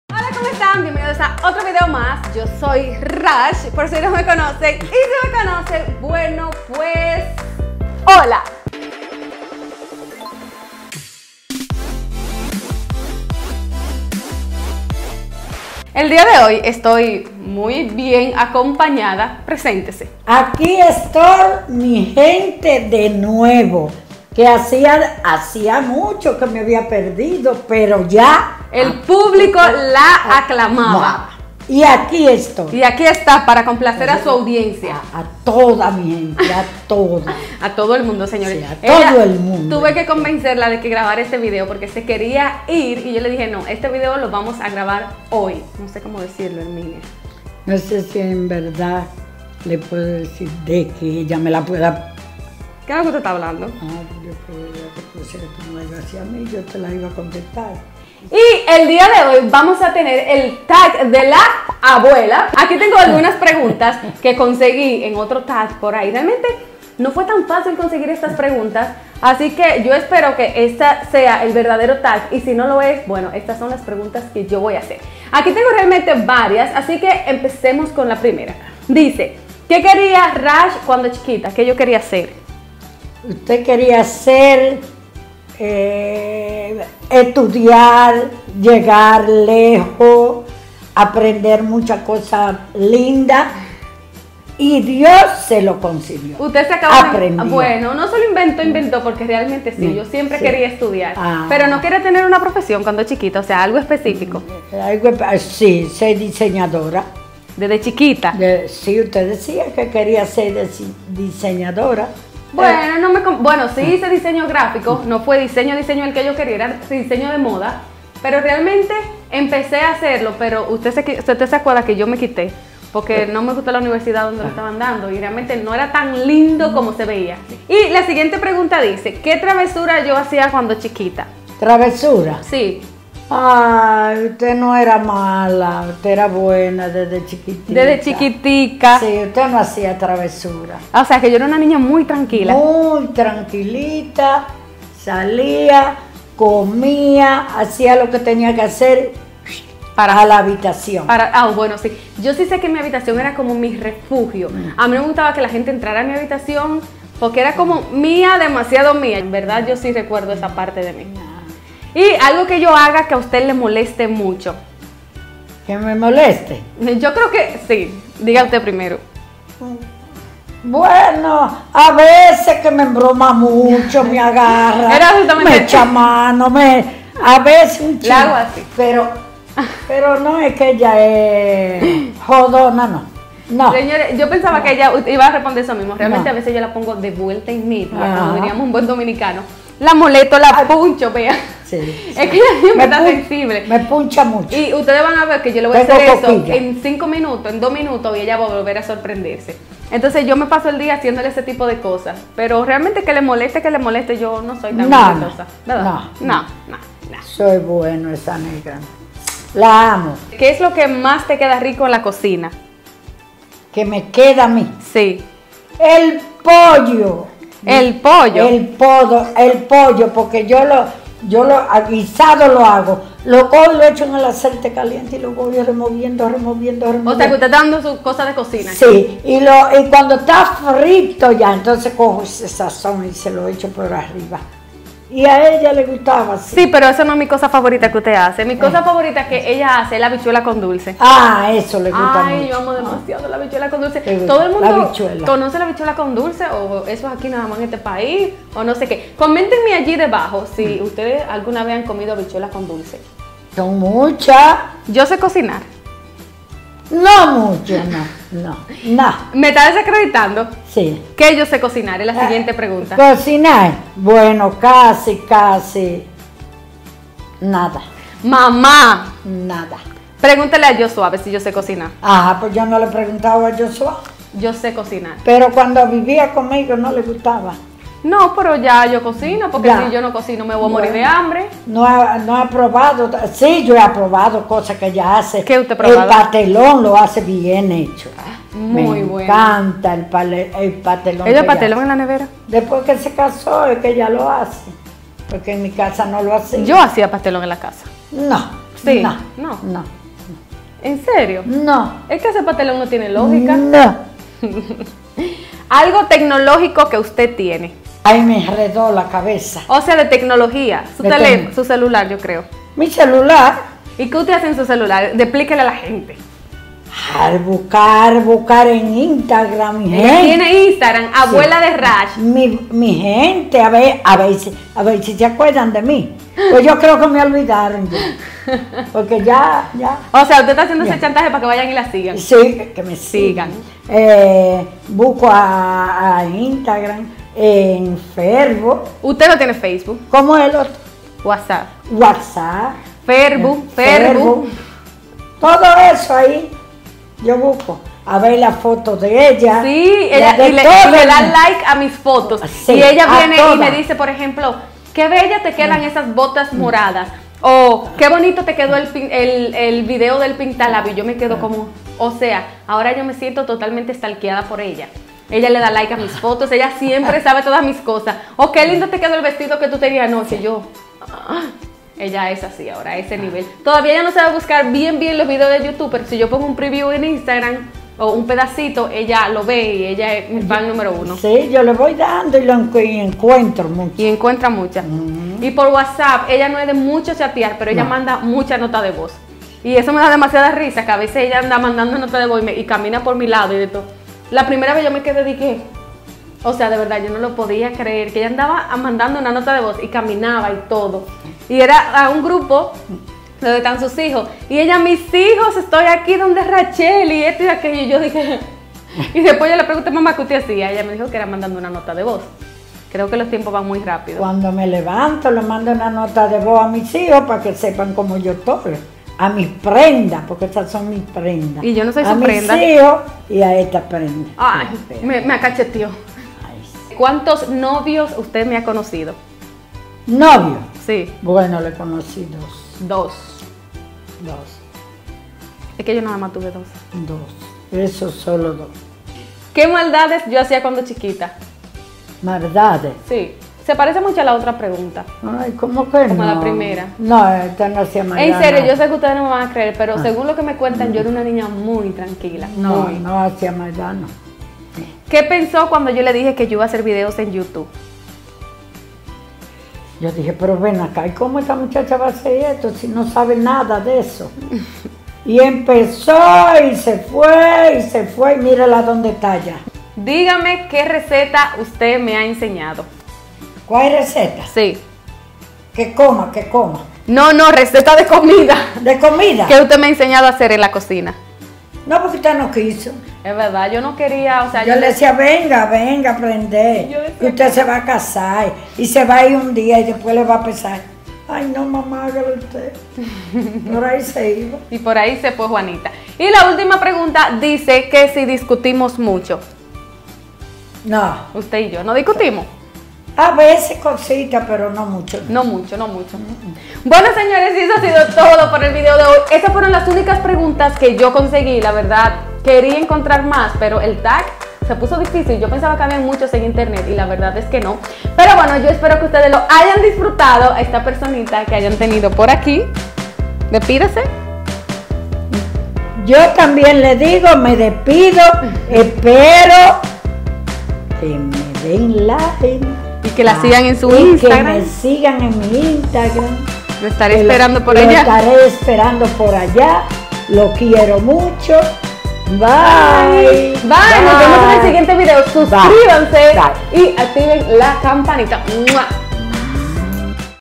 TAG DE LA ABUELA 👵🏼💕 | RACHZX
HOLA CHIQUILLOS AQUI ANDO CON UNA INVITDA QUE UDS QUIEREN MUCHOTE MI ABUELA, ESPERO QUE DISFRUTEN Y RECUERDEN QUE LOS QUIERO MUCHOOOOTE!

♥ s i g u e m e ♥  y  hablemos un poco!